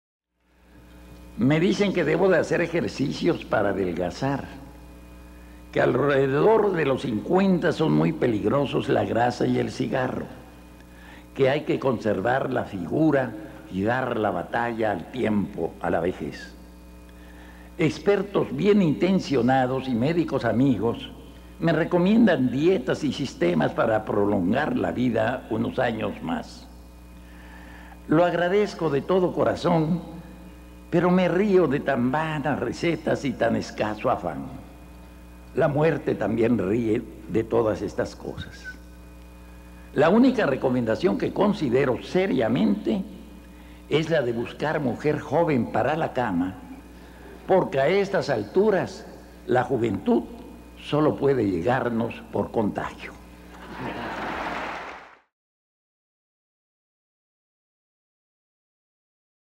POESÍA: JAIME SABINES. Pensándolo bien.
Poesía de Jaime Sabines leída en su última aparición pública en Chiapas en 1997.

Jaime Sabines Gutiérrez (Tuxtla Gutiérrez, Chiapas,1926-Ciudad de México; 1999) fue un poeta y político chiapaneco, reconocido como uno de los grandes poetas mexicanos del siglo XX.

Me dicen que debo hacer ejercicio para adelgazar,
que alrededor de los 50′s son muy peligrosos la grasa y el cigarro,
que hay que conservar la figura
y dar la batalla al tiempo, a la vejez.

Expertos bien intencionados y médicos amigos
me recomiendan dietas y sistemas
para prolongar la vida unos años más.

Lo agradezco de todo corazón, pero me río
de tan vanas recetas y tan escaso afán.
(La muerte también ríe de todas esas cosas.)

La única recomendación que considero seriamente
Es la de llevar una mujer joven a la cama
Porque a estas alturas, la juventud
Solo puede llegarme por contagio.